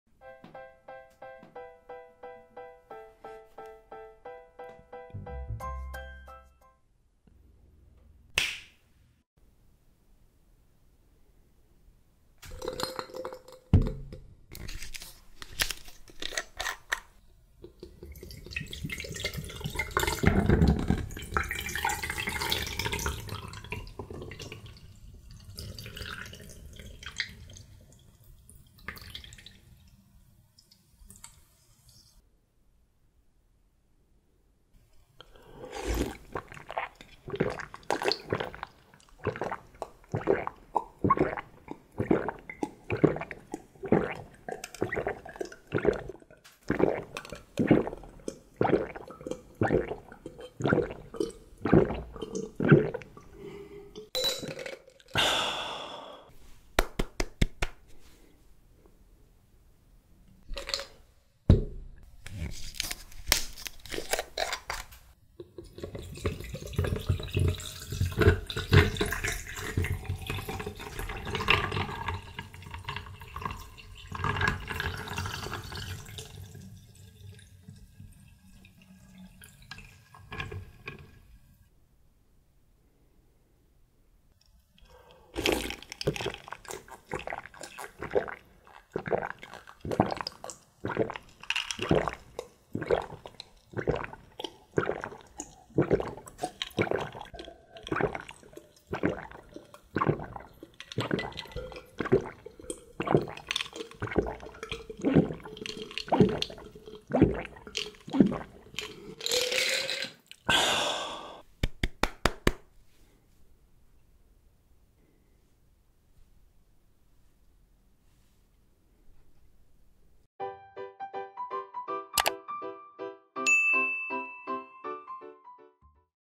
편의점 음료수 추천 👍 이렇게 맛있어도 되나? 쟈뎅 아워티 2종 원샷 먹방 ASMR Drinking Sound
⭐오늘의 원샷⭐

👉 쟈뎅 아워티 2종
👉 레몬 얼그레이티 & 오렌지 자몽 블랙티
👉 500ml x 2

새로 나온 신상 음료 리뷰부터
수분보충 갈증해소, 숙취해소까지!
시원한 얼음 음료 원샷 & 먹방으로
ASMR 대리만족감을 팍팍 드려요😉

✔파트너스 활동을 통해 자곰이 일정액의 수수료를 제공받을 수 있어요

*자막은 설정에서 선택후 보실 수 있어요
*영상이 흐린 경우 설정에서 1080p를 눌러주세요